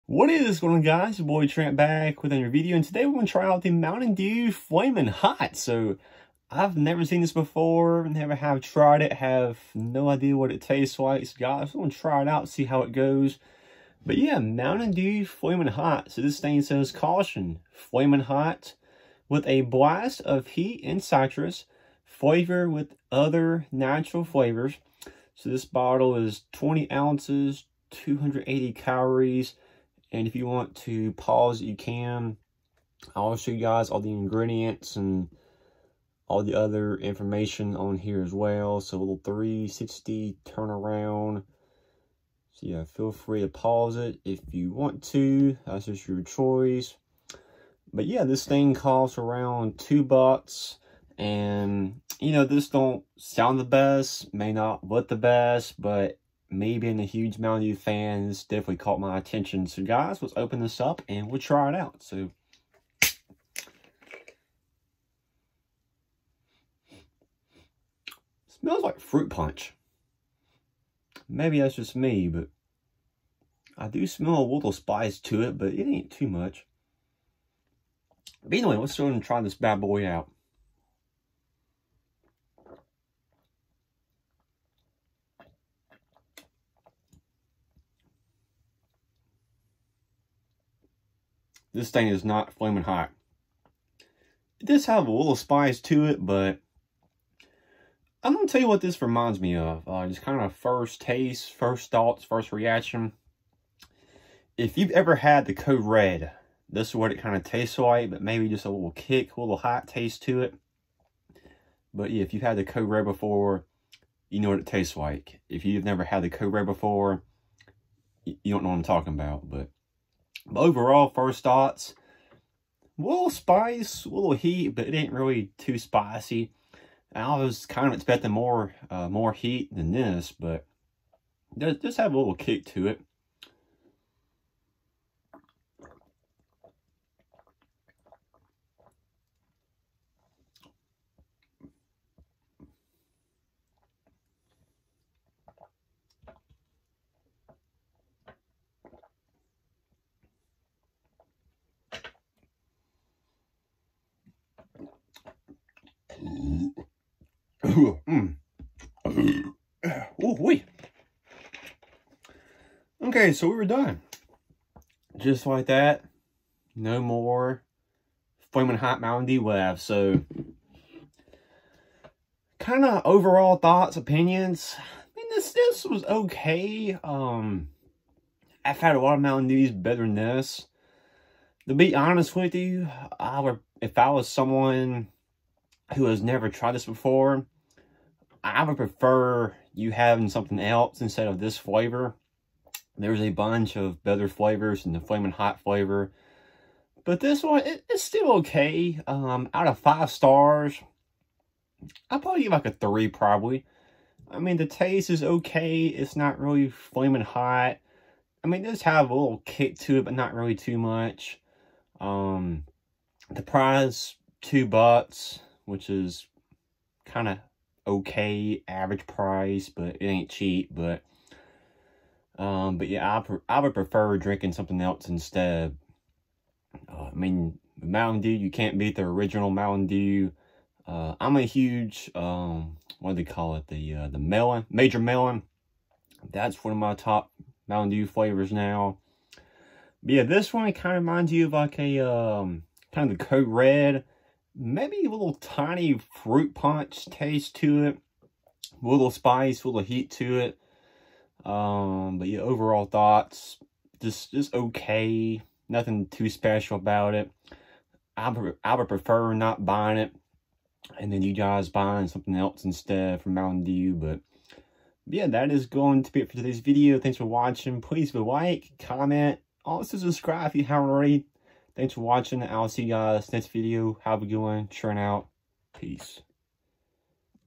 New!!!! Mountain Dew Flamin’ Hot Review. Spicy Taste Test Review
Today we try the BRAND NEW Mountain Dew Flamin’ Hot Soda. This bottle is a 20 OZ Bottle and 280 calories. Cost me 2 dollars at a local store in my town

Have you tried the new Mountain Dew Flamin Hot Soda and what’s your thoughts and opinions? Also what’s your favorite Mountain Dew?

Thanks again for watching and have a good day peeps